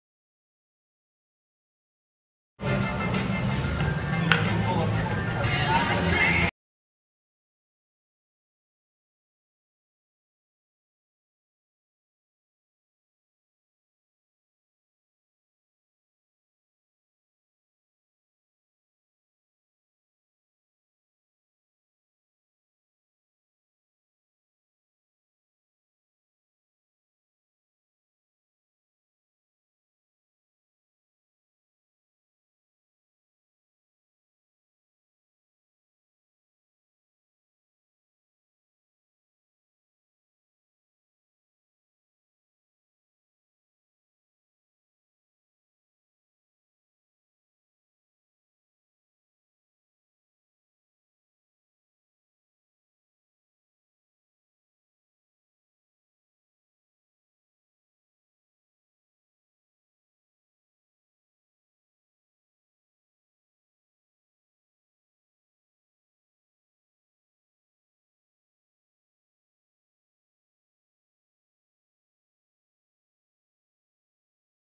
Andy Pettitte throws BP for Yankees
Yankees great Andy Pettitte, in town for last weekend's 1996 World Series reunion, threw batting practice on Monday at Yankee Stadium.